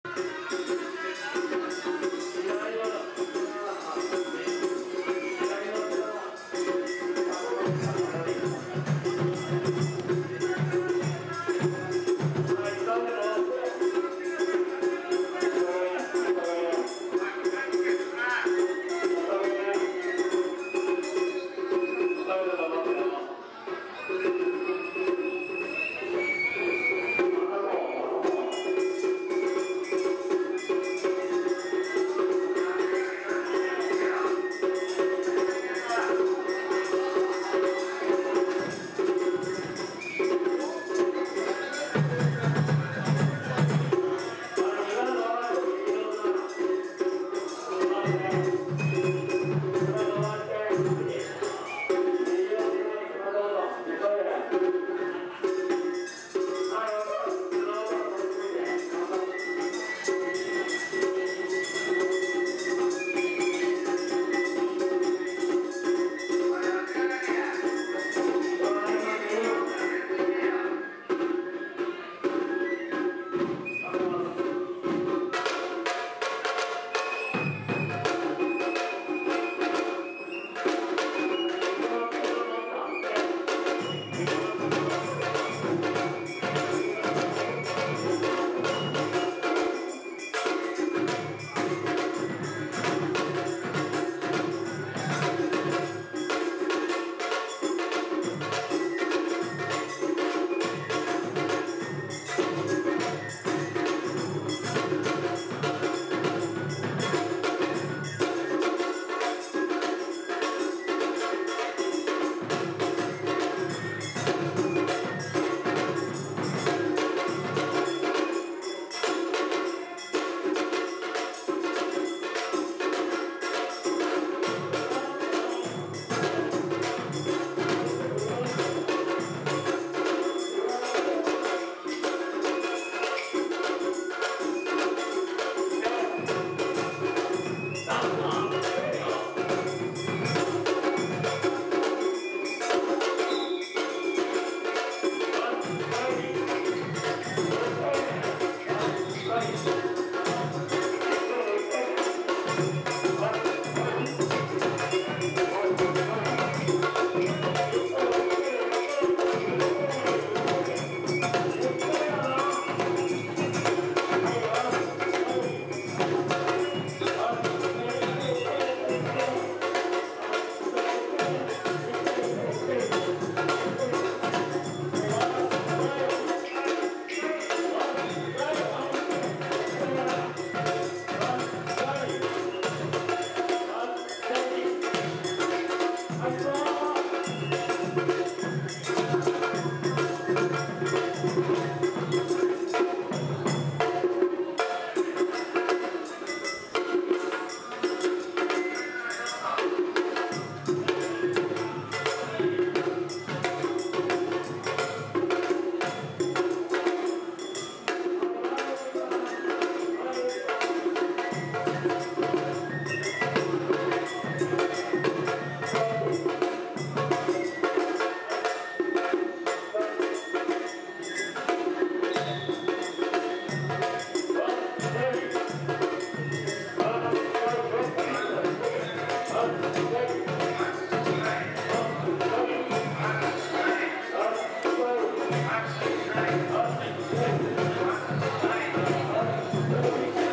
仲之町の山車 3日目曳き廻しスタートです！ 成田祇園祭2022楽日にて 2022年7月10日 00043
成田祇園祭2022楽日にて
2022年7月10日
千葉県成田市